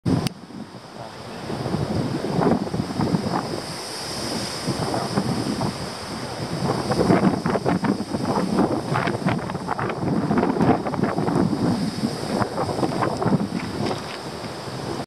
Pic Paradis Movie Moment
March 18, 2017 a short clip of the view from Pic Paradis on St. Martin.